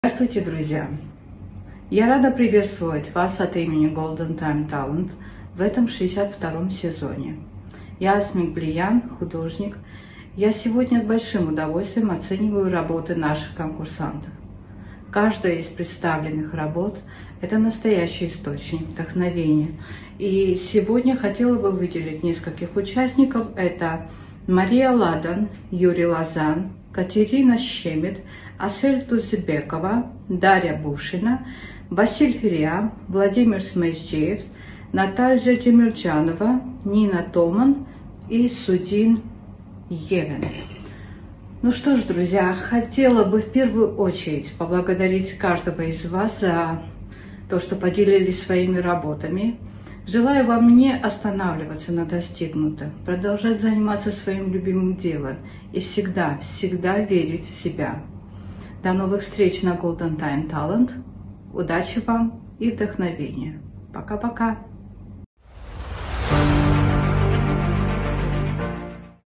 Video evaluation of season 62 - Golden Time Talent jury - Asmik Bleyan 🇬🇪
Dear friends!
We publish a video assessment of the 62 season from the jury of the Golden Time Talent contest - Asmik Bleyan 🇬🇪 Georgia